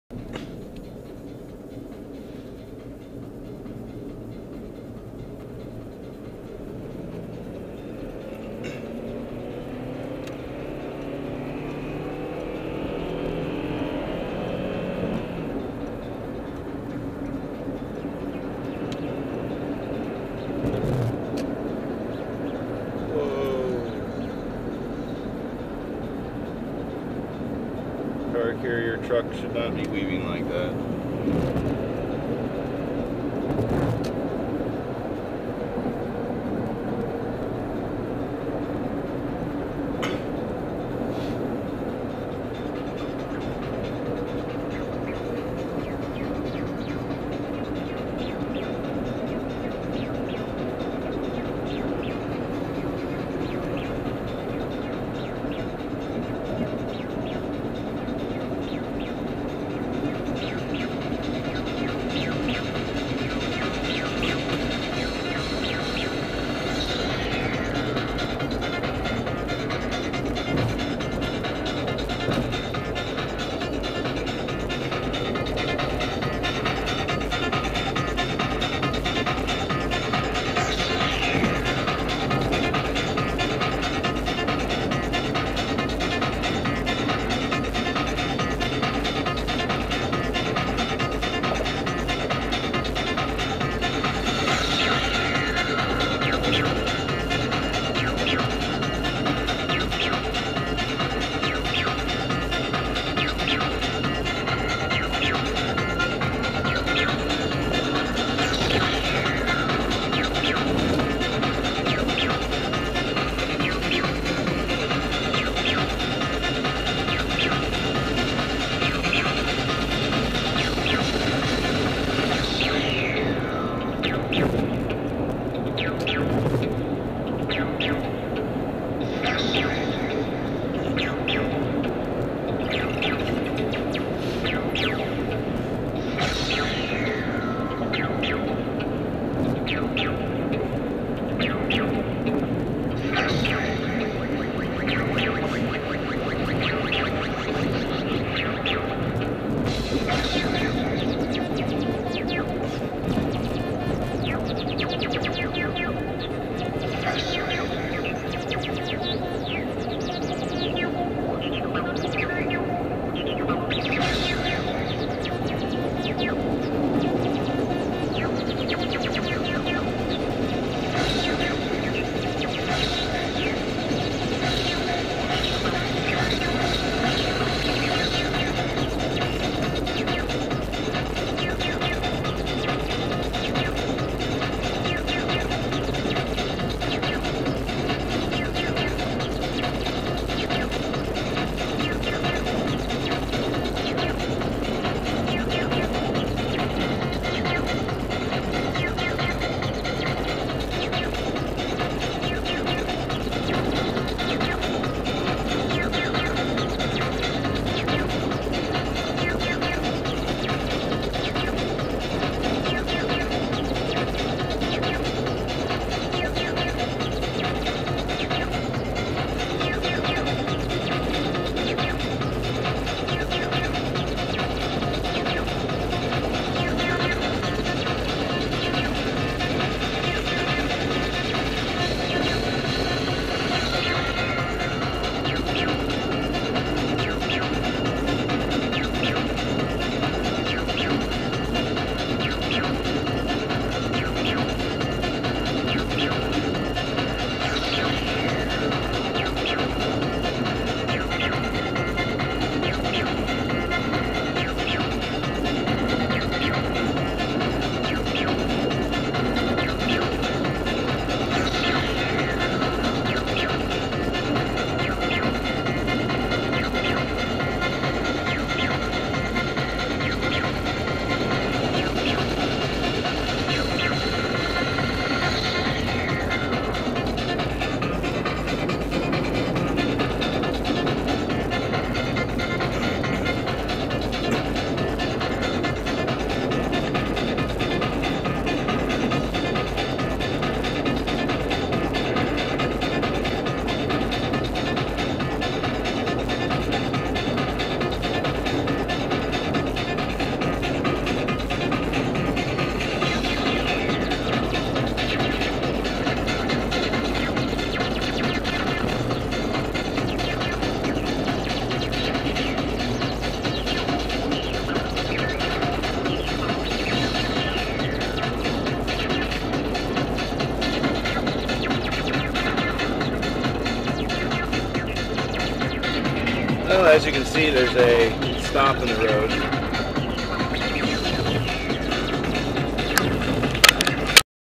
2011.11.17 Daily Driving: Irvine Medical Cannabis Renewal [2/10]
Drove to Irvine today for my medical cannabis renewal. Driving around now with MORE commentary! Sometimes even special Guests. Just an example video from my Contour HD camera to show off the video quality to friends. You might want to turn down your speakers, the microphone is lacking.